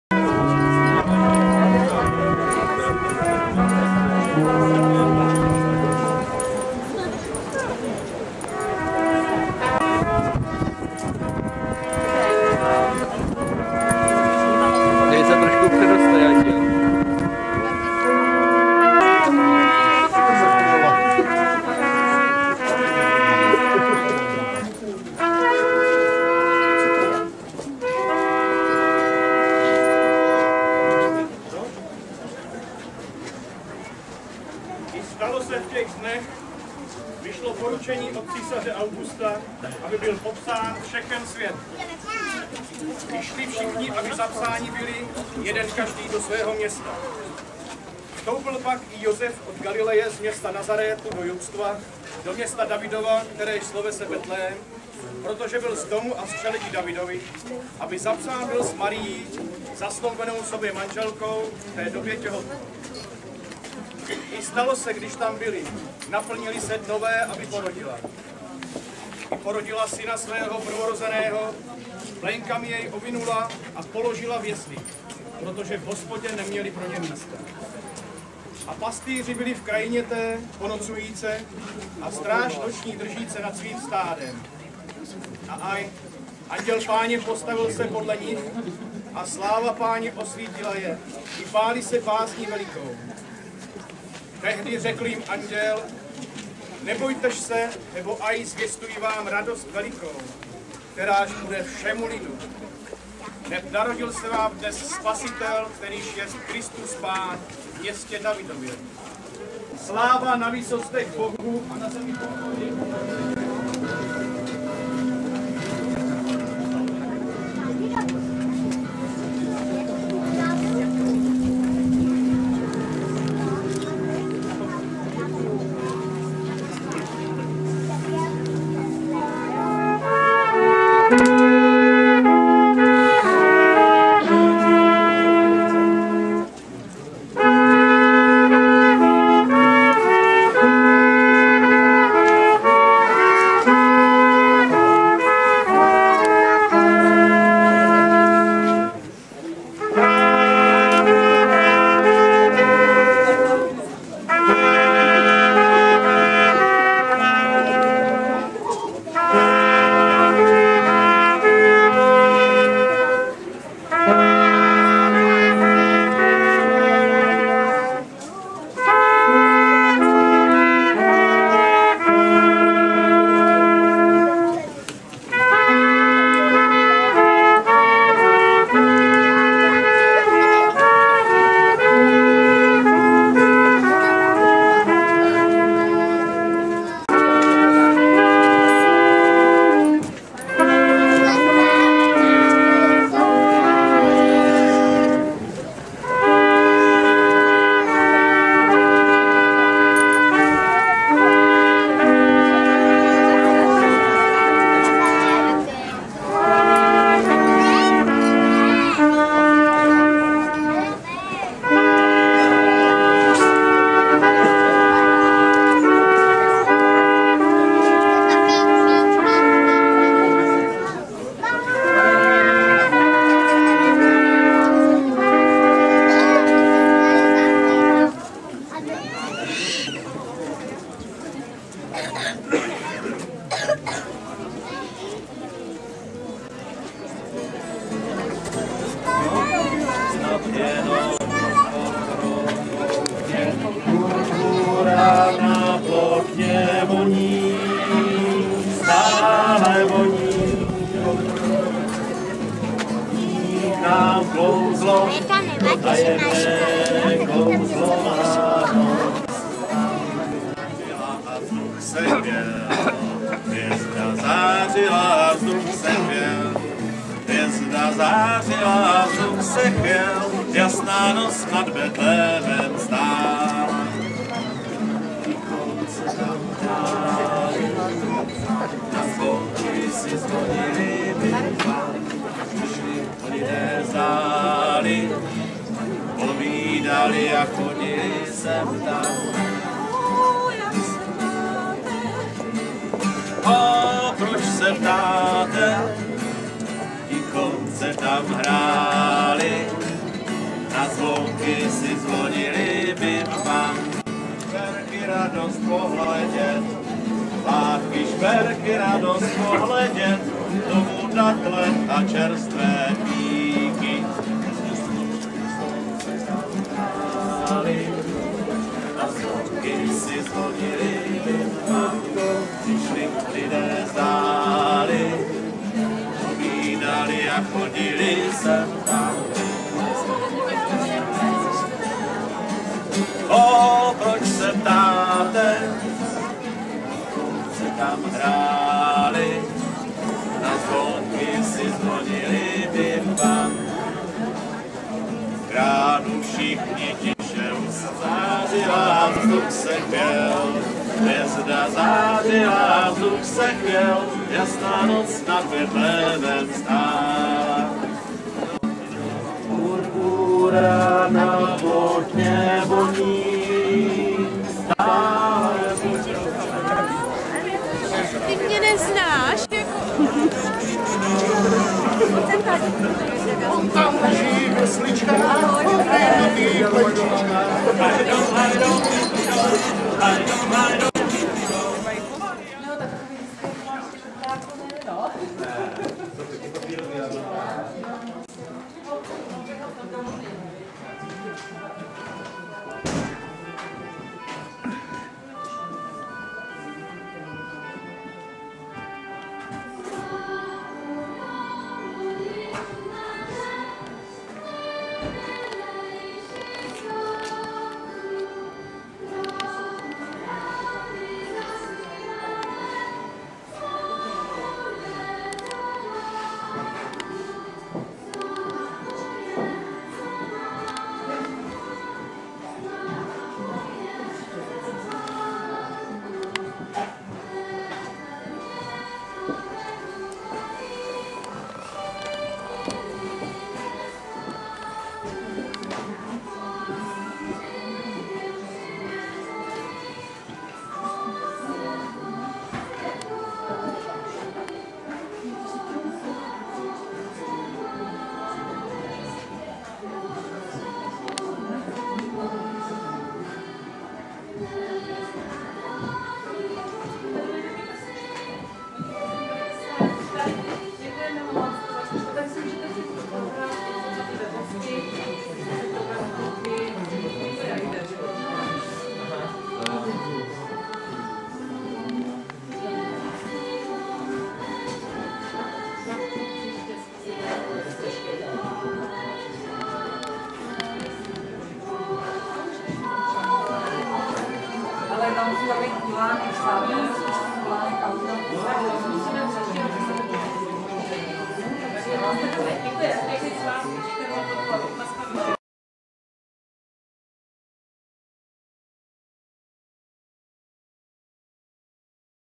Živý betlém - Záměl
Živý betlém s rozdáváním betlémského světla. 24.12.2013 Záměl
Pořadatel Má vlast - můj domov o.s.